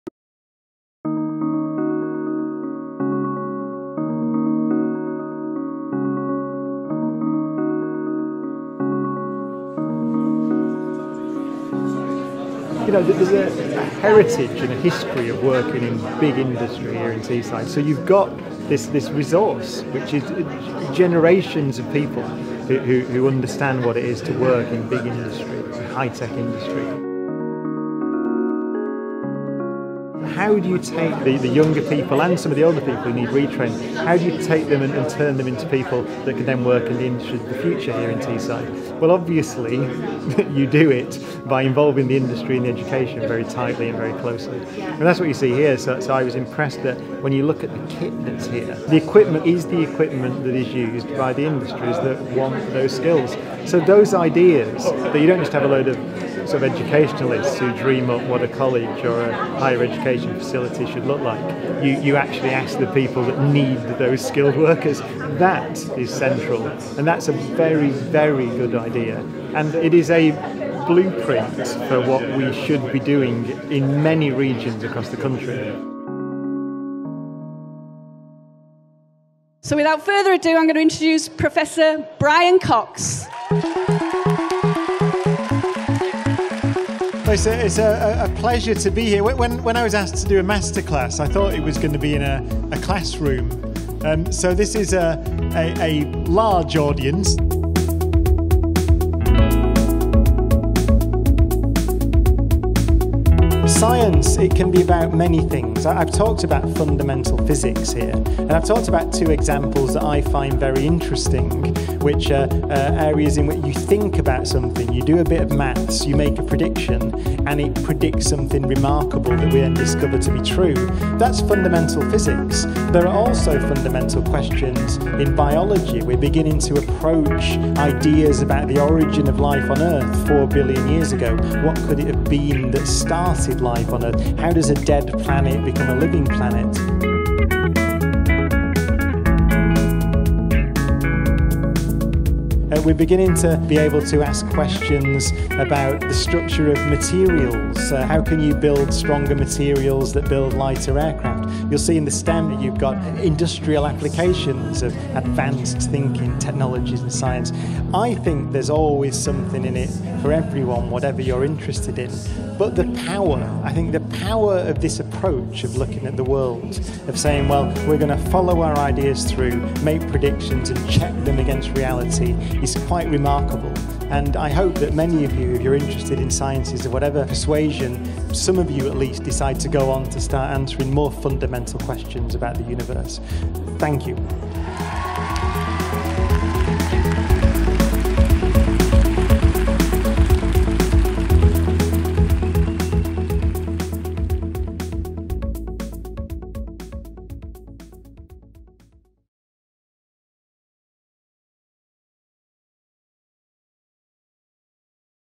Professor Brian Cox opens Middlesbrough College STEM Centre
Professor Brian Cox OBE visits Middlesbrough College to open the new £20m STEM skills training centre, describing it as "a blueprint for what we should be doing in many regions across the country".

Watch as Professor Cox officially opens the centre and wows students with an impressive physics masterclass.